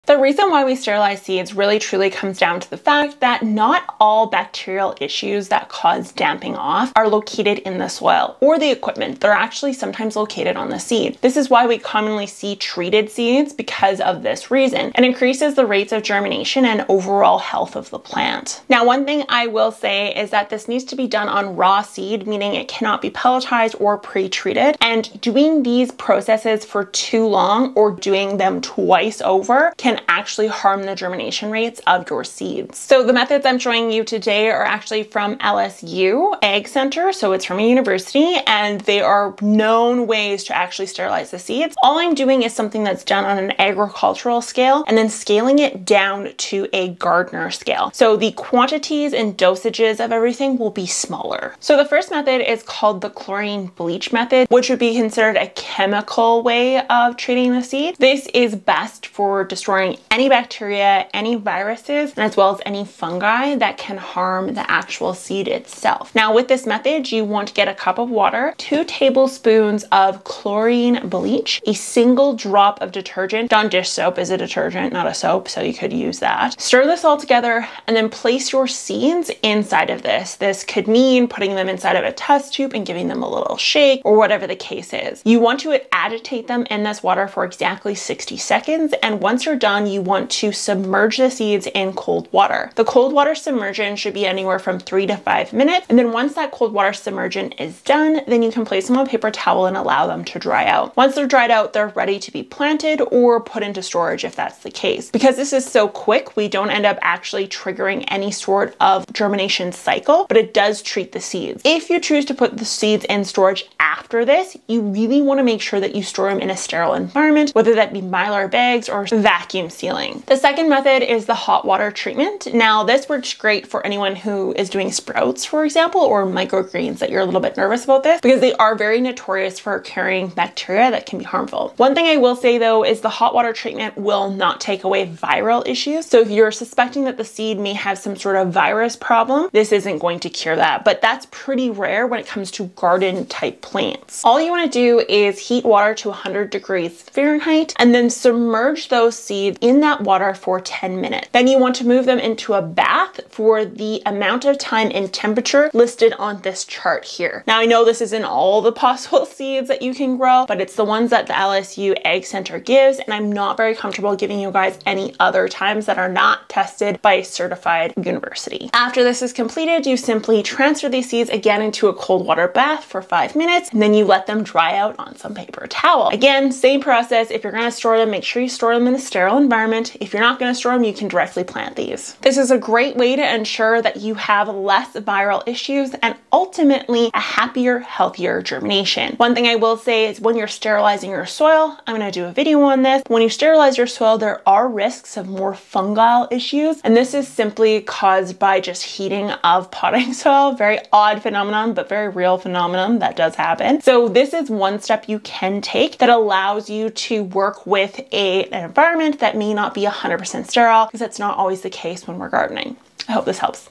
How To Sterilize Seeds To Prevent Bacteria & Mold. Dampening Off Fix & Perfect For Microgreens.
Paperback Houseplant Planner:
Paperback Garden Planner:
Digital Download Planners